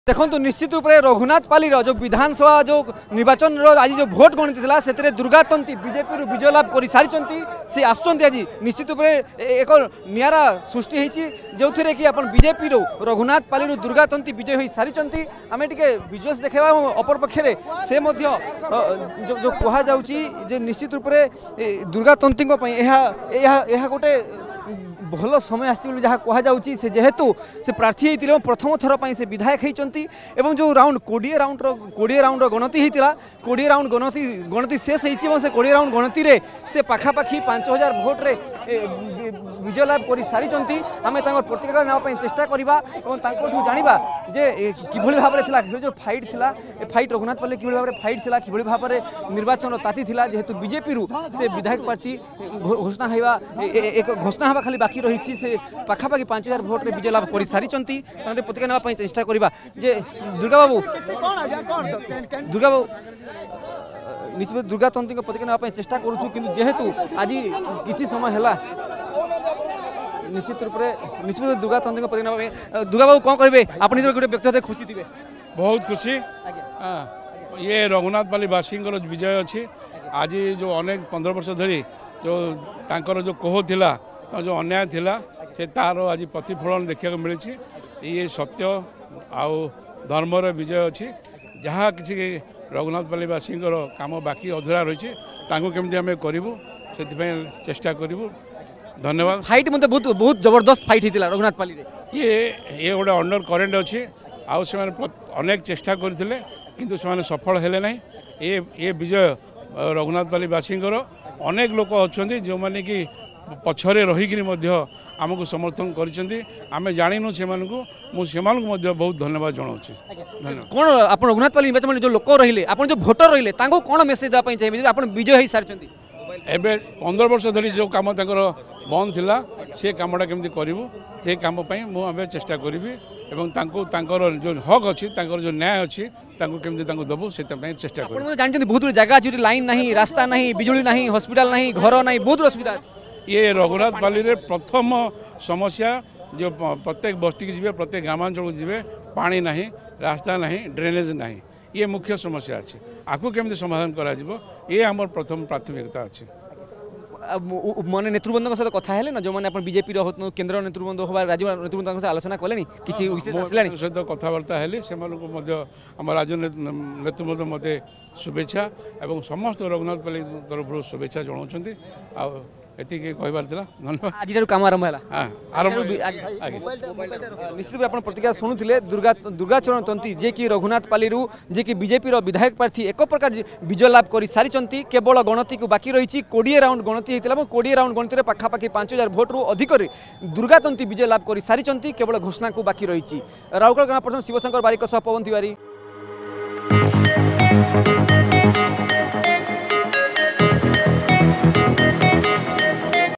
ରଘୁନାଥପାଲିରୁ ବିଜେପି ପ୍ରାର୍ଥୀ ଦୁର୍ଗା ତନ୍ତି ବିଜୟୀ ହୋଇଛନ୍ତି |Election Result 2024 |BJP |Odisha Reporter
ରଘୁନାଥପାଲିରୁ ବିଜେପି ପ୍ରାର୍ଥୀ ଦୁର୍ଗା ତନ୍ତି ବିଜୟୀ ହୋଇଛନ୍ତି । ବିଜୟ ପରେ ଦୁର୍ଗା କଣ କହୁଛନ୍ତି ଶୁଣନ୍ତୁ.